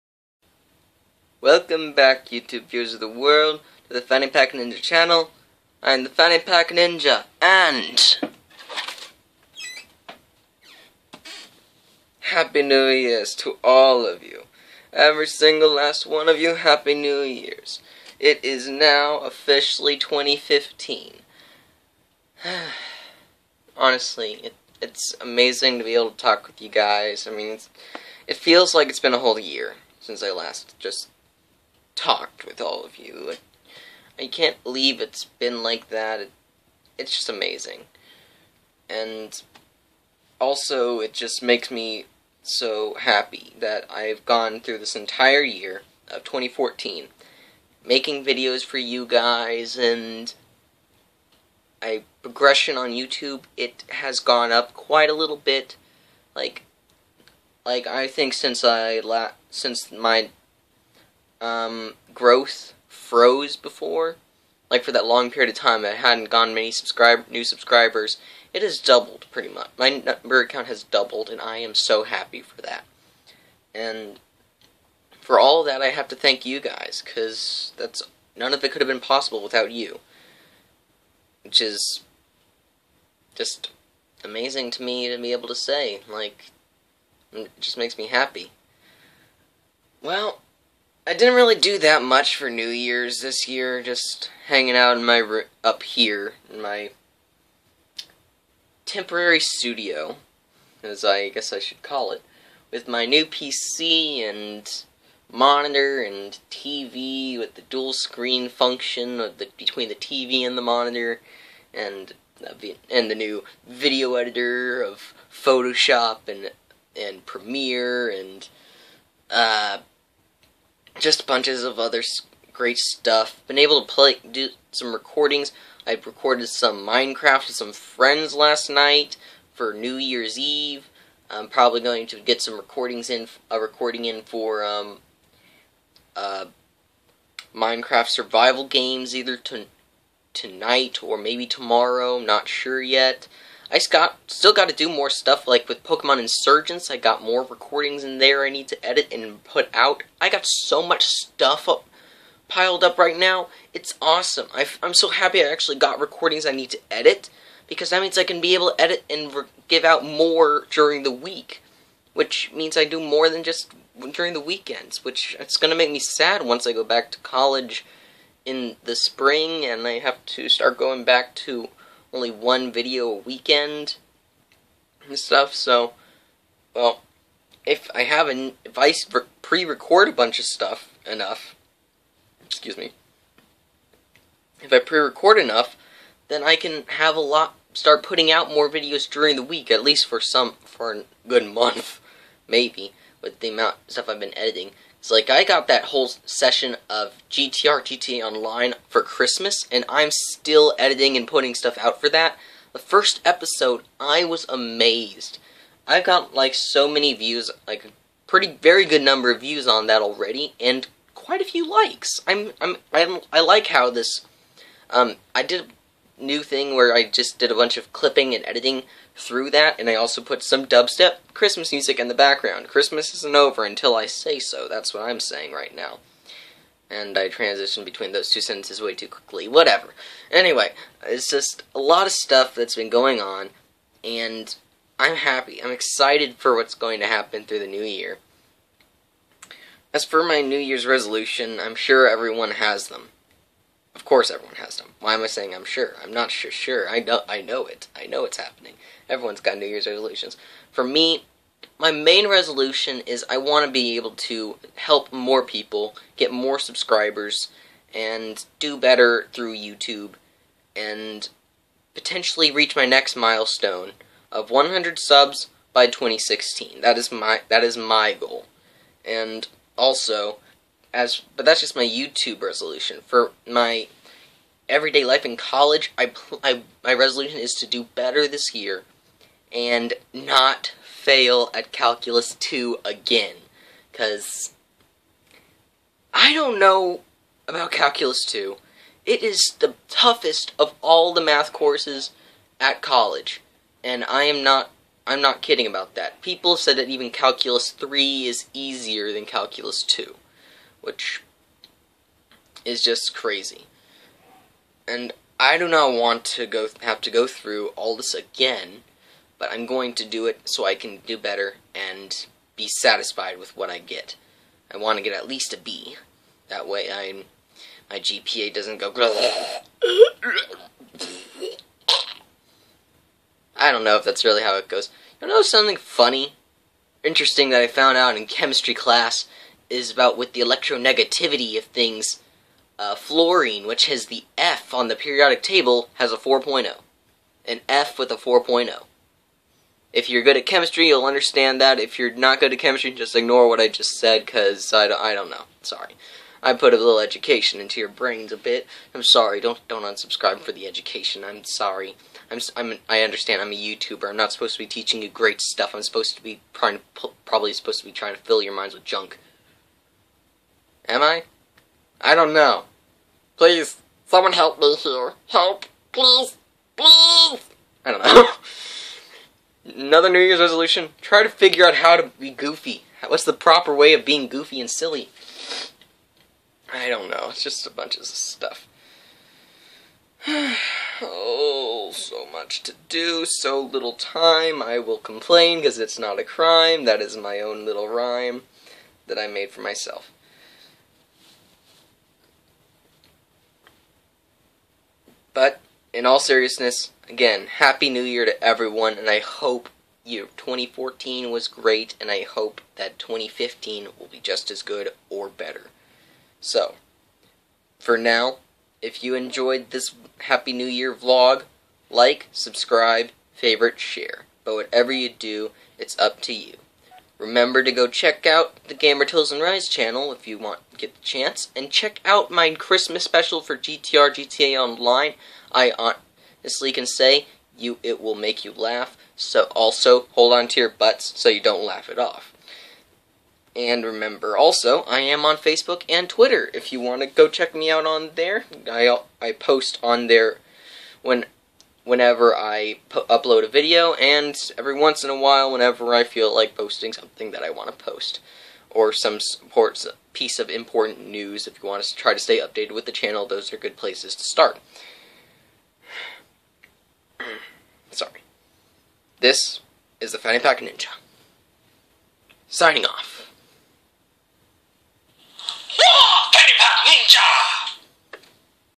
Fanny Pack Ninja - Happy New Year 2015
"It has been a great honor to make videos for you this past year. I laughed, I cried, I grew, and I loved every minute of it. Now, I must say farewell, because I will no longer be making videos for you. Good bye..."
Fanny Pack Ninja of 2014

Now that that is out of the way, GREETINGS EVERYONE! I am the Fanny Pack Ninja of 2015! Let's make this the best year ever!

Curious about why I'm the Fanny Pack Ninja? Checkout my first video. It has the answer you are looking